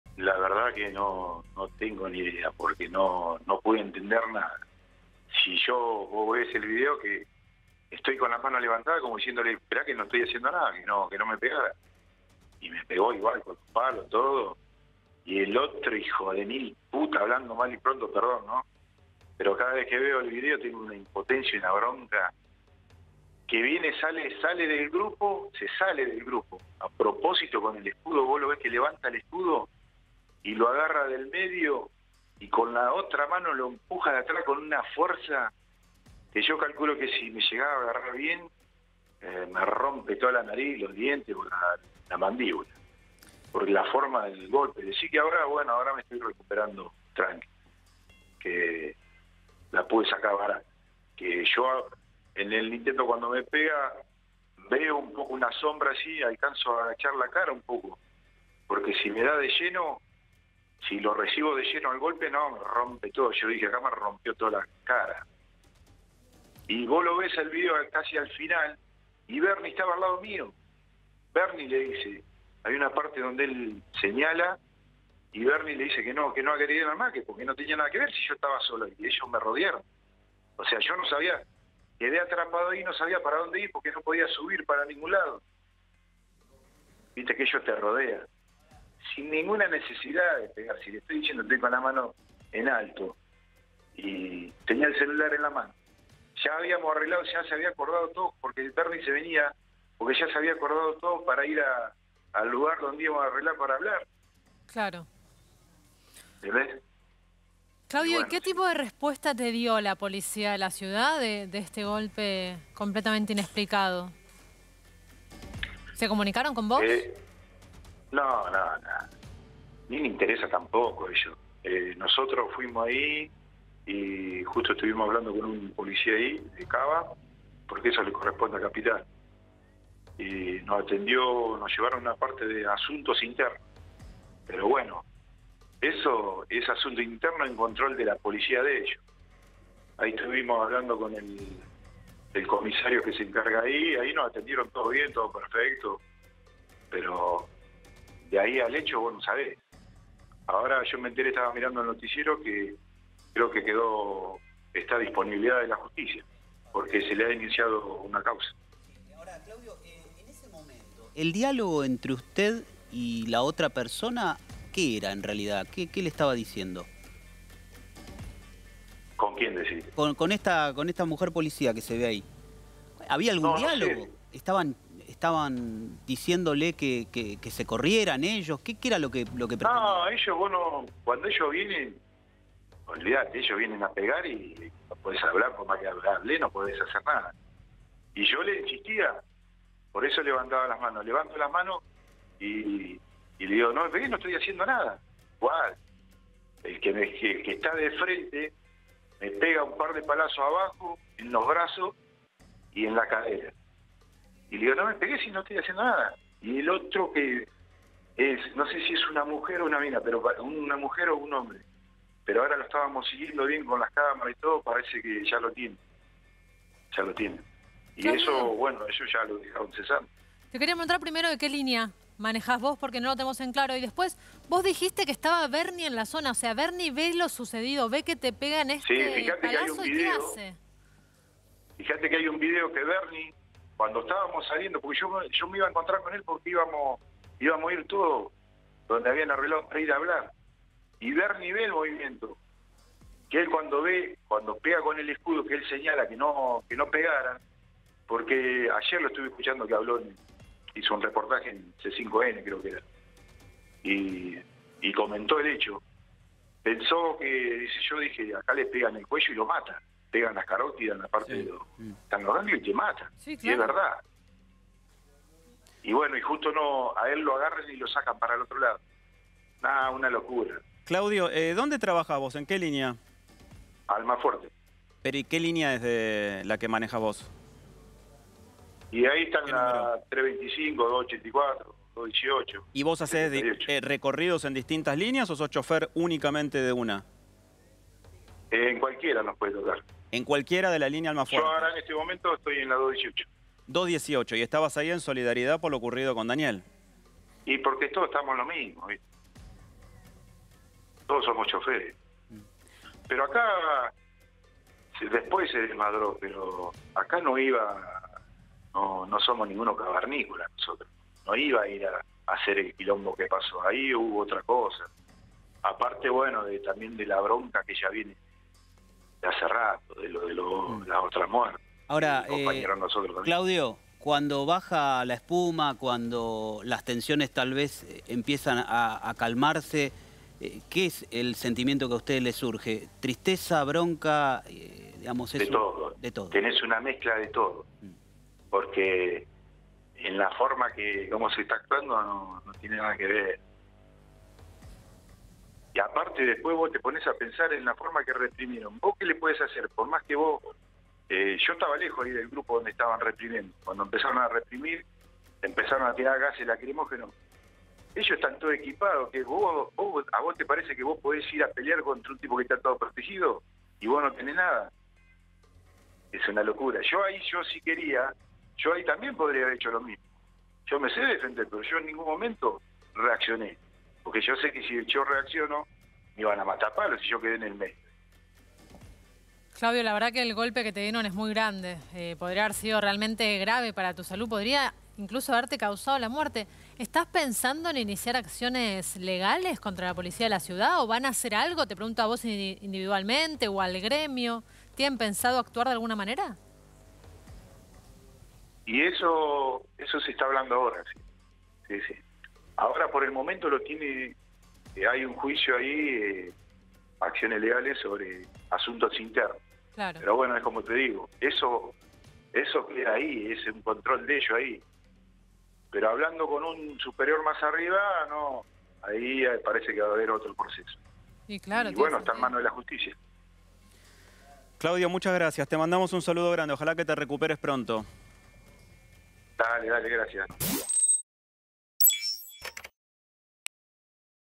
"Le pedí que no me pegara y me pegó igual": el chofer agredido por el policía de CABA
Claudio Barchessi, el hombre agredido por el policía de la Ciudad, afirmó que el efectivo "se salió del grupo" para "pegarle": "Tengo una impotencia bárbara", concluyó.

✅ SEGUÍ A IP NOTICIAS EN